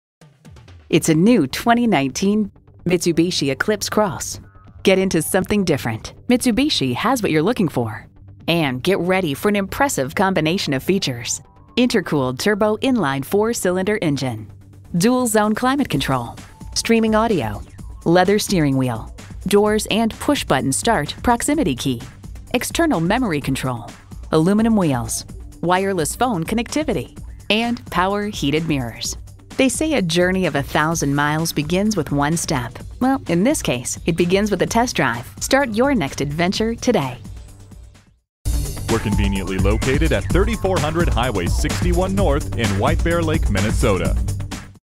New 2019 Mitsubishi Eclipse Cross Saint Paul White-Bear-Lake, MN #93997 - SOLD
Year: 2019
Make: Mitsubishi
Model: Eclipse Cross
Trim: SEL S-AWC
Engine: 91 L 4 Cyl. Cyl.
Transmission: Automatic
Color: Pearl White
Interior: BLACK
Stock #: 93997
VIN: JA4AT5AAXKZ021561

Address:
3400 Highway 61 North